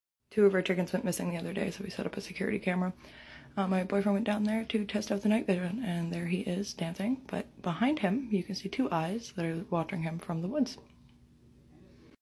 WTF did i just watch, Security camera outdoor footage, Strange
Security camera catches outdoor oddity from a strange individual